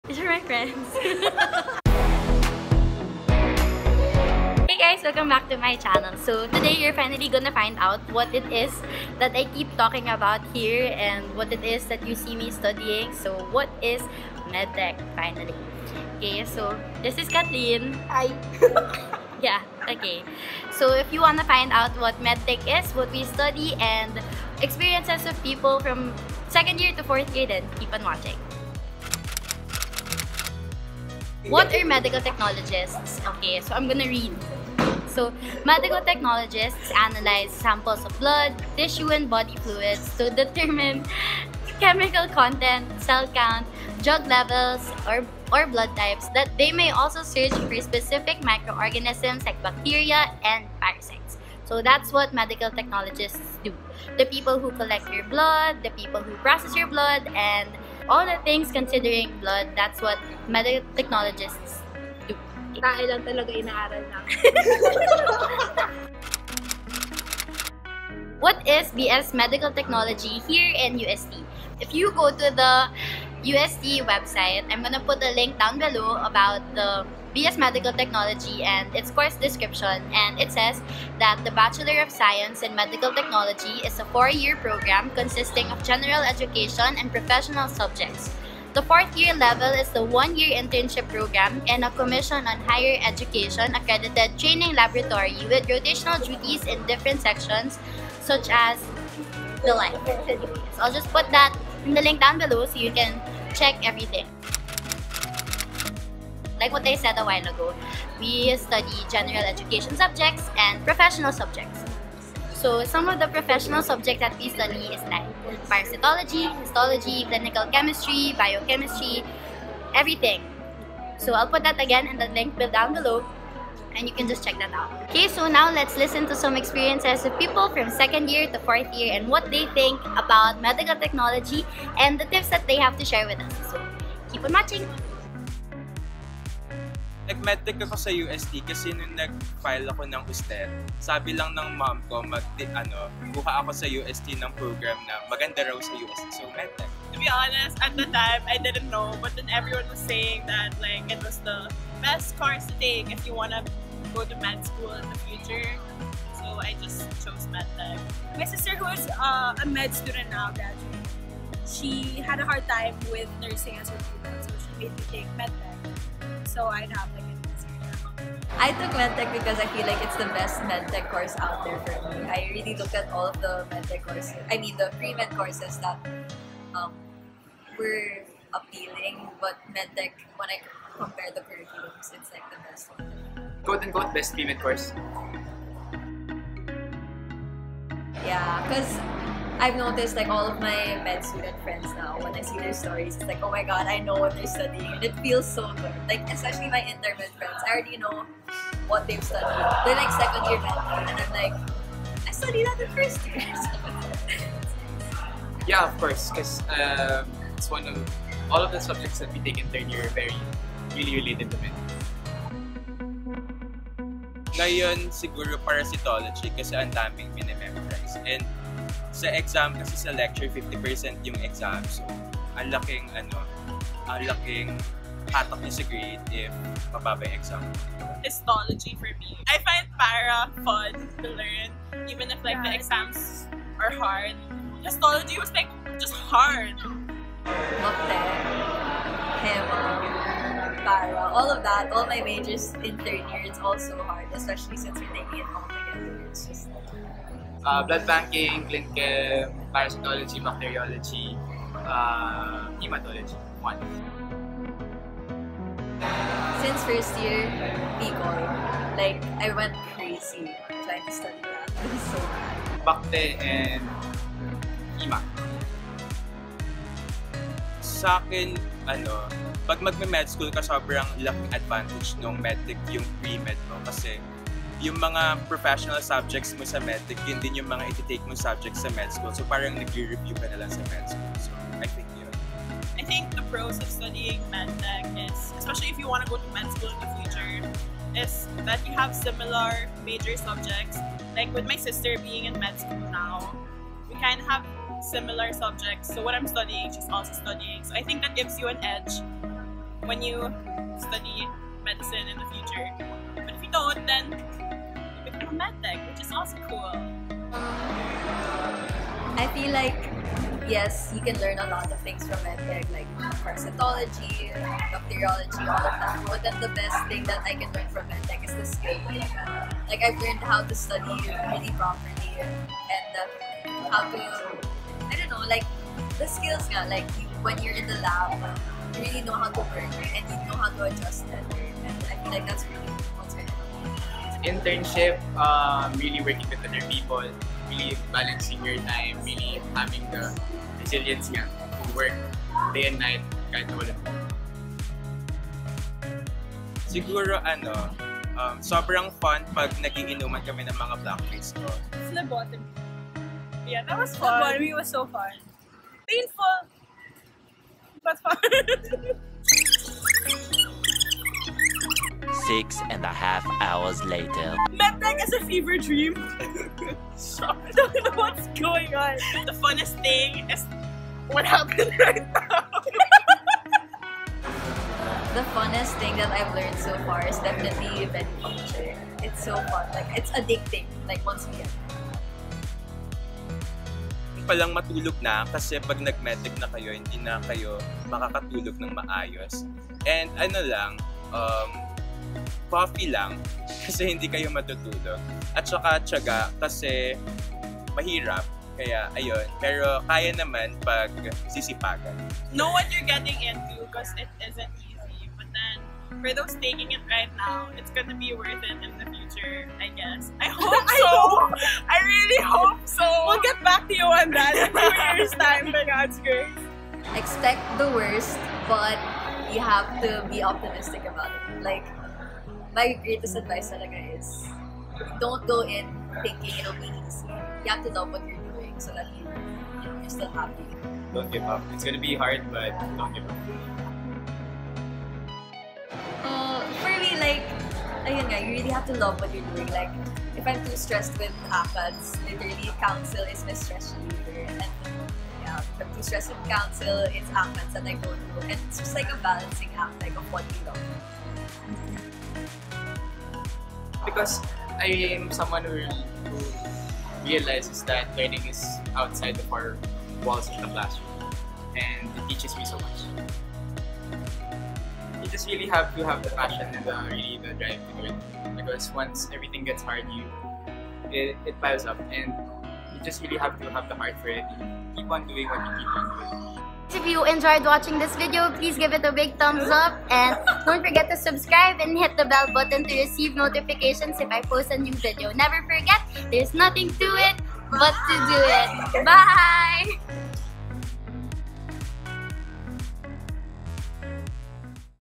What is MedTech? Hear from the students! 👩🏻‍🔬 | Medical Technology (Philippines)
After so long hearing "Med Tech", you finally get to find out what it is we really do! 💛

Watch this and find out what are Medical Technologists, what do we study and hear some experiences of MT students on how to survive 🤓

Thank you so much Renz, Jeanne, Yara, Lia, and Enzo for filming this video with me! Much love!

I really hope you guys loved watching this video! ☺️  Don't forget there's NOTHIN TO IT BUT TO DO IT ❤️ I’ll see you guys in my next video!!